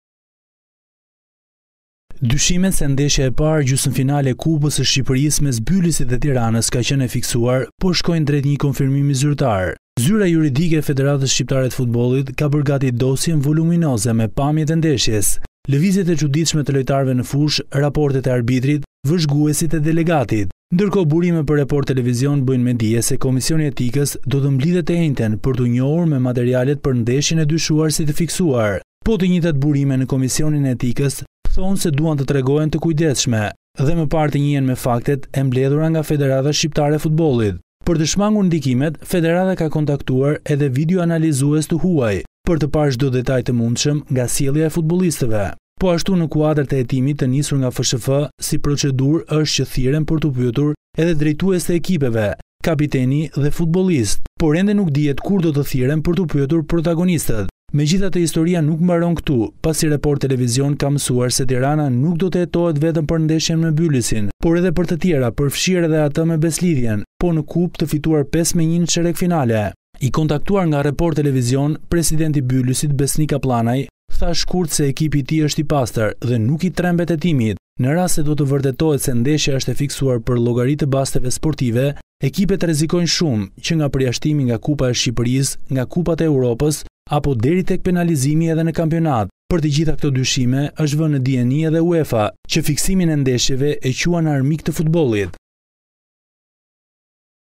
Report TV -Bylis-Tirana u manipulua? Komisioni i Etikës të enjten merr në shqyrtim dosjen voluminoze
TIRANË- Dyshimet se ndeshja e parë gjysmëfinale e Kupës së Shqipërisë mes Bylisit dhe Tiranës ka qënë e fiksuar po shkojnë drejt një konfirmimi zyrtar. Zyra juridike pranë FSHF-së ka bërë gati dosjen voluminoze me pamjet e ndeshjes, lëvizjet e cuditshme të lojtarëve në fushë ,raportet e arbitrit, vëzhguesit e delegatit. Burime për Report Tv bëjnë me dije se Komisioni i Etikës do të mblidhet të enjten për t'u njohur me materialet për ndeshjen e dyshuar si të fiksuar. Po të njëjtat burime ne Komisionin e Etikës thonë se duan të tregohen të kujdesshëm dhe më parë të njihen me faktet e mbledhura nga FSHF.